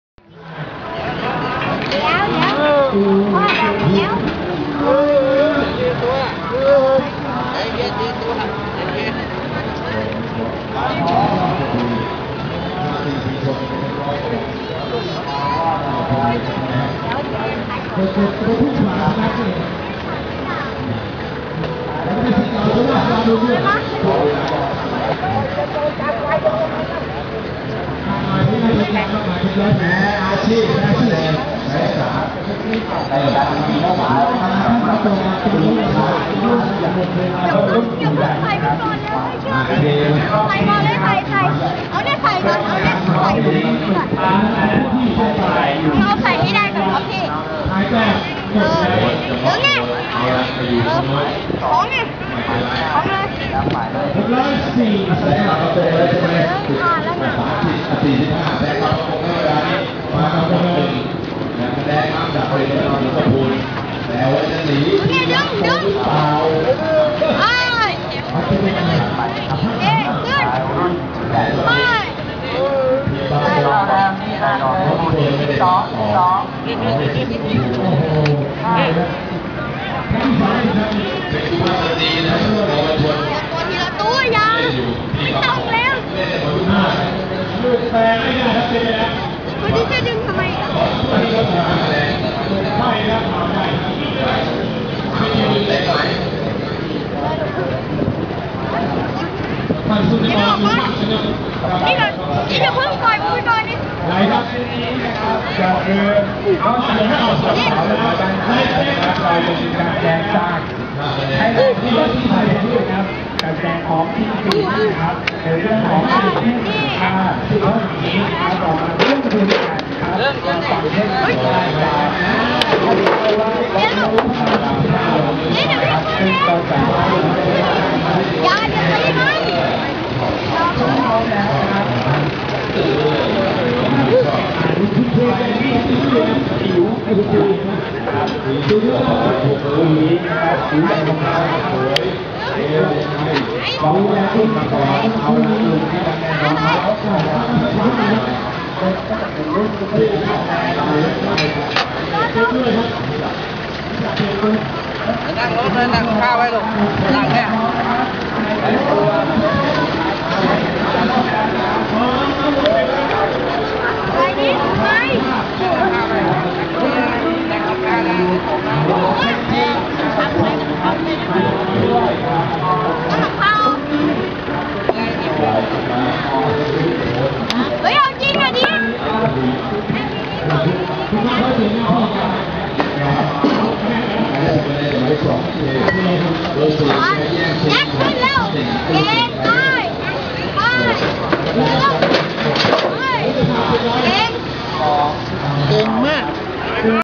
141 ปี วิ่งควาย ชลบุรี 2/3
ส่งเจ้าควายเผือกขึ้นรถกลับบ้าน